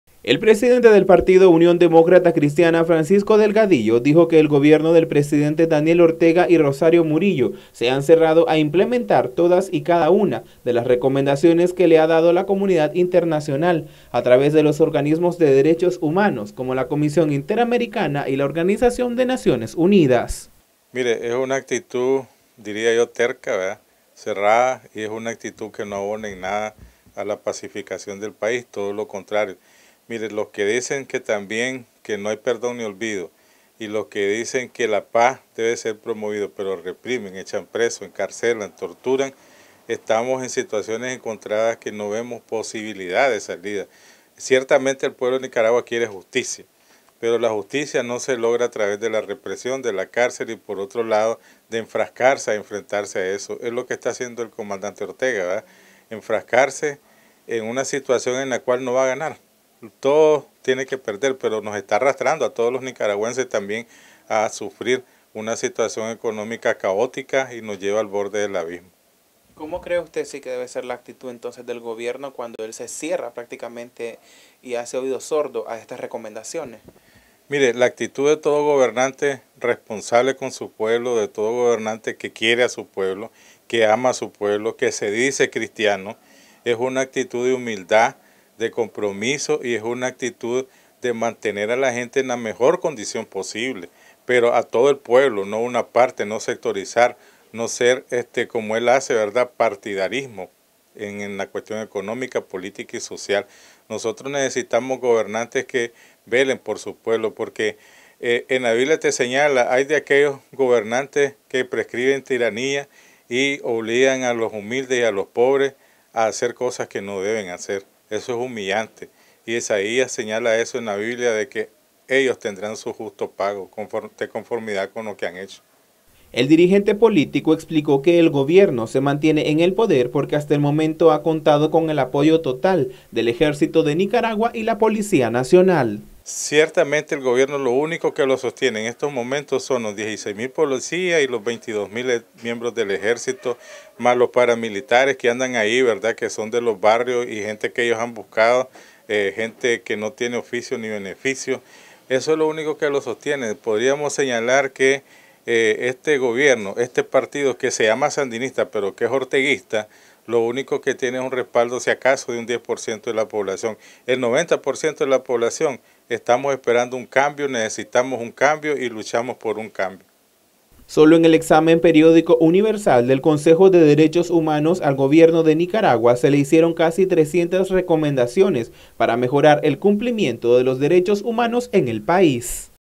“ORTEGA SE CIERRA A RECOMENDACIONES DE LA COMUNIDAD INTERNACIONAL”
Dirigentes políticos dicen que el Gobierno de Daniel Ortega se cierra a las recomendaciones de la comunidad internacional.